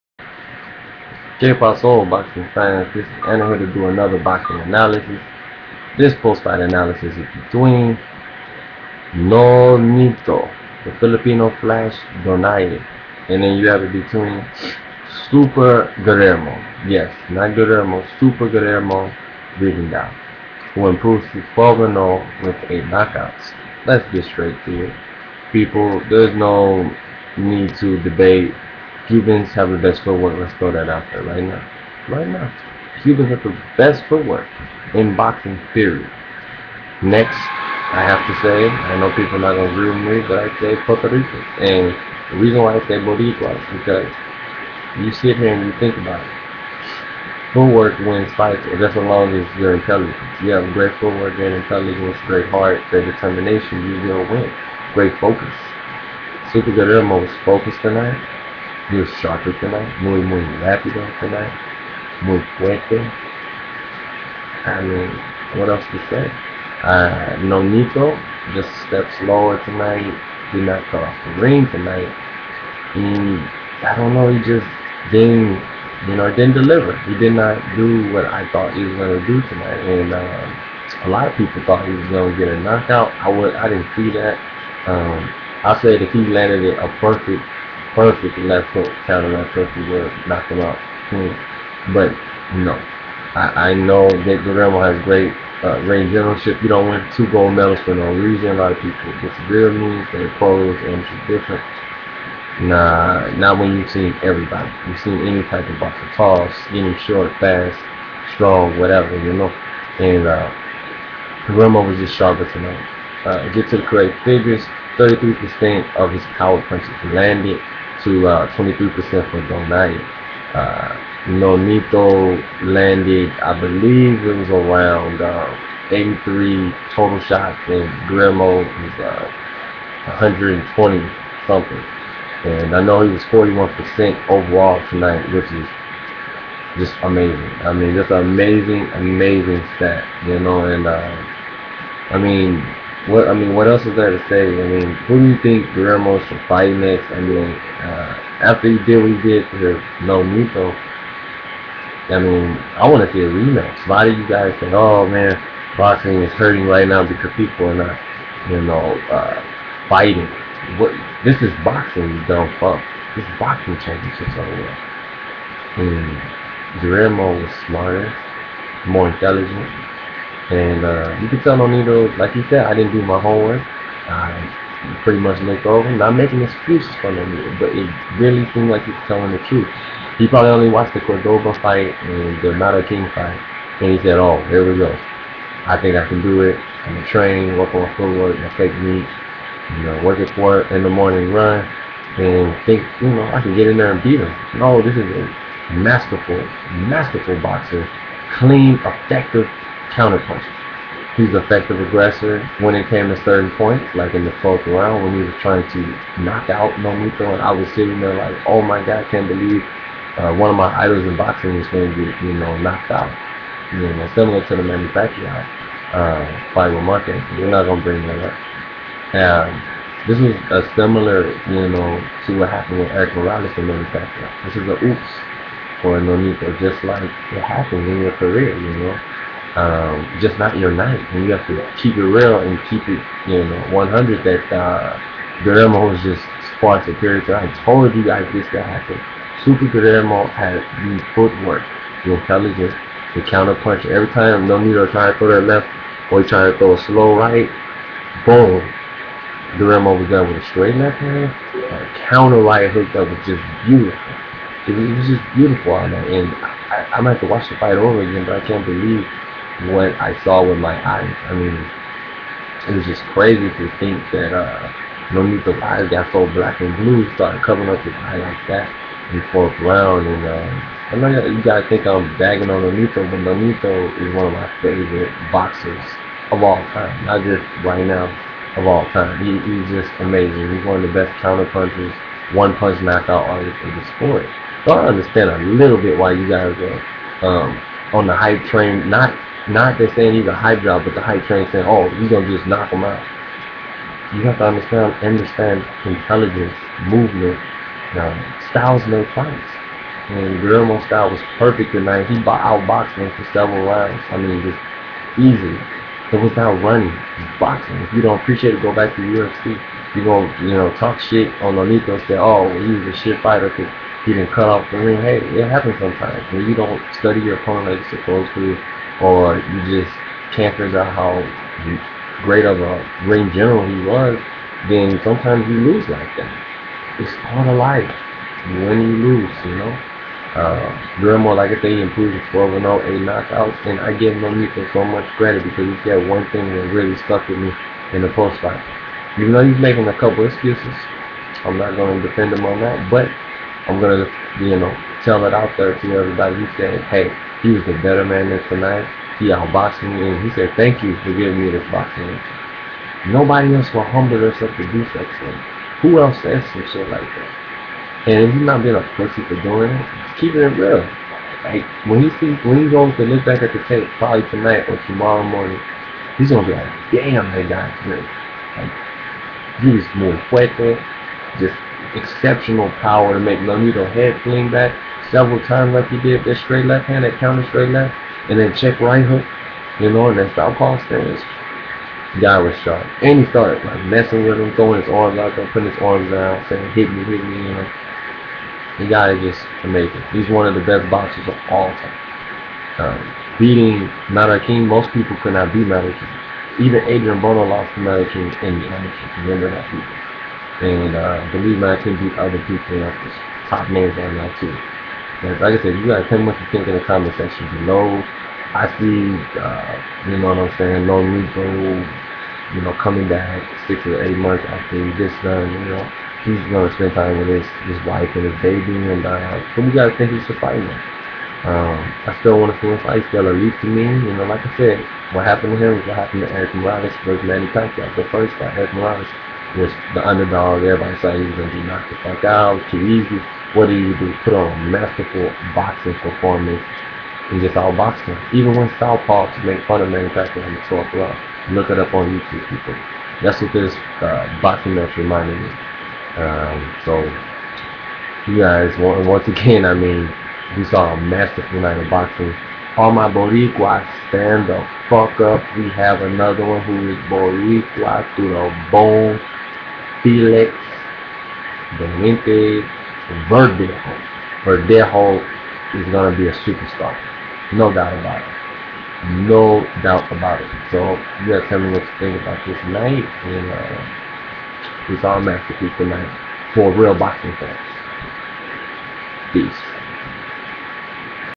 DONAIRE VS RIGONDEAUX POST FIGHT ANALYSIS! SUPER GUILLERMO RIGONDEAUX IS AMAZING!
RIGONDEAUX GAVE NONITO A BOXING LESSON